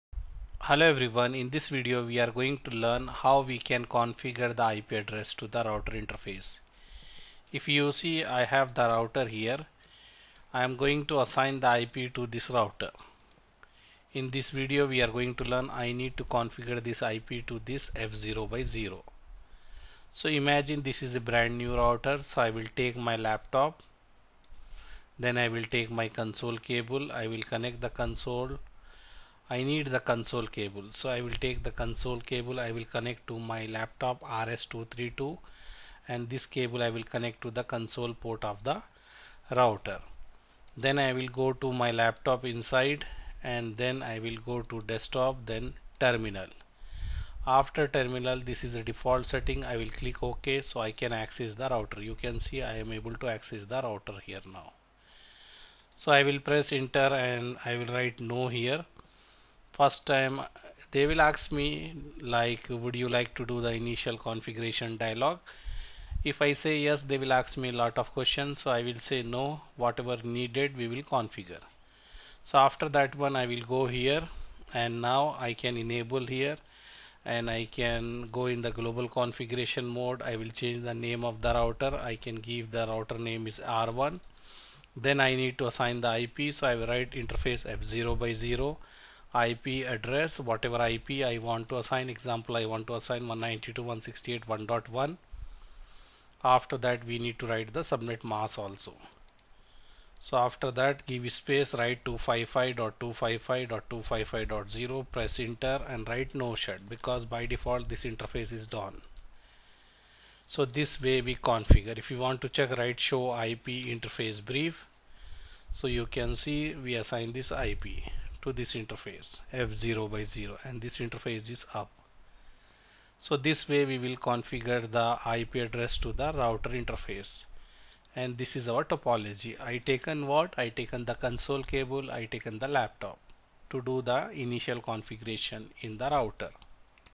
How to Assign IP address to Brand New Router with Console?
Hello Everyone,

In this video we are going to discuss How to Assign IP address to Brand New Router with Console?
Thanks.

About us:
Thanks